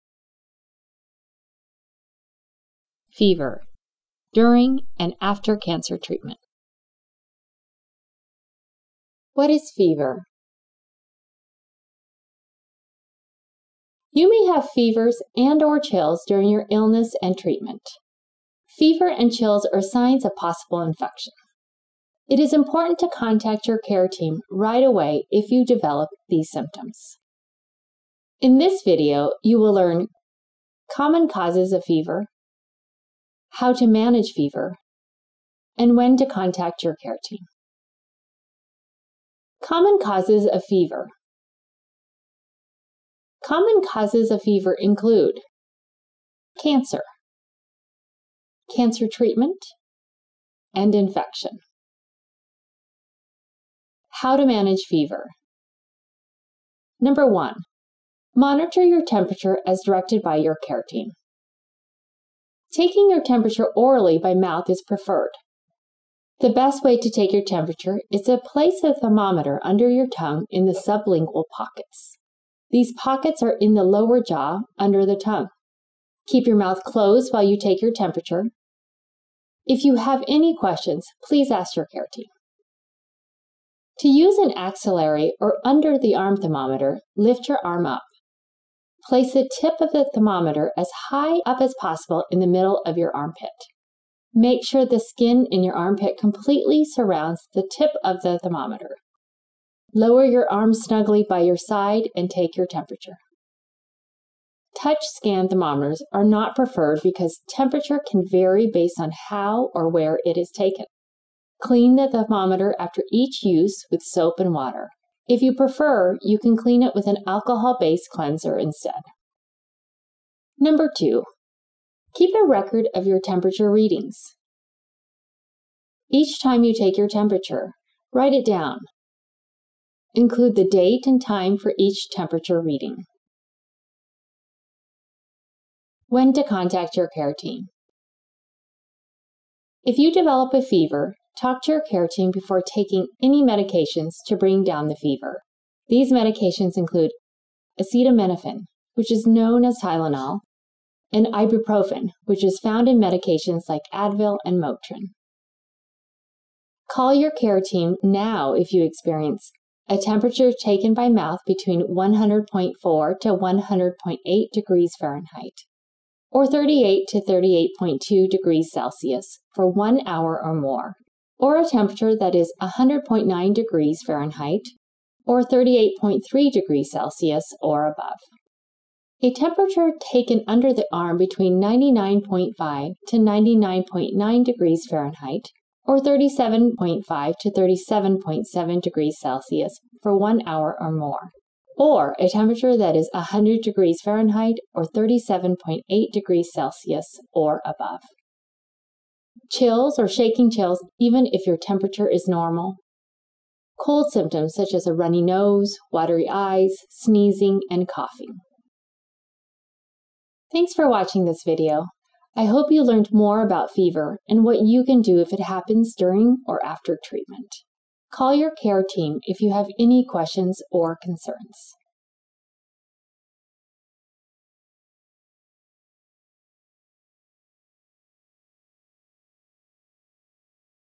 Fever: During and After Cancer Treatment
Fever is a common side effect of cancer and its treatment. In this video, you will learn:

- What is fever
- Common causes of fever
- How to manage fever
- When to contact your care team